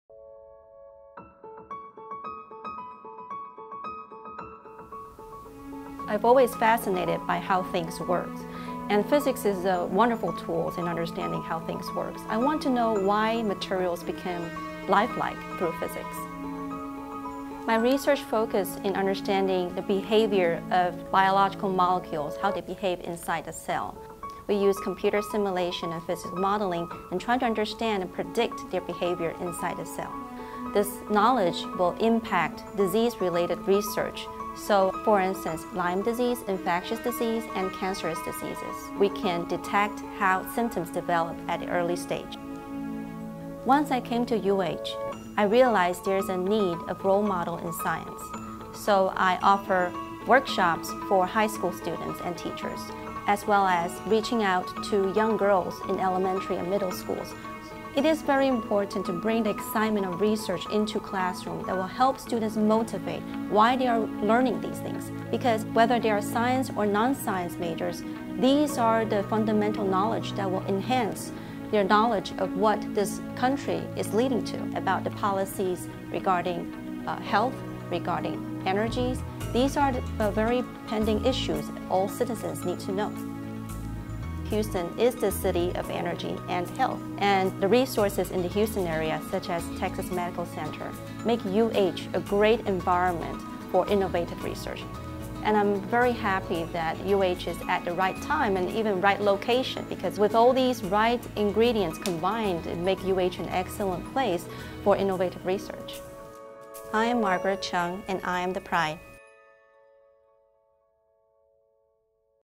You Are the Pride: Margaret Cheung
UH physics professor Margaret Cheung discusses her passion for mentoring and explains why UH is a great environment for innovative research.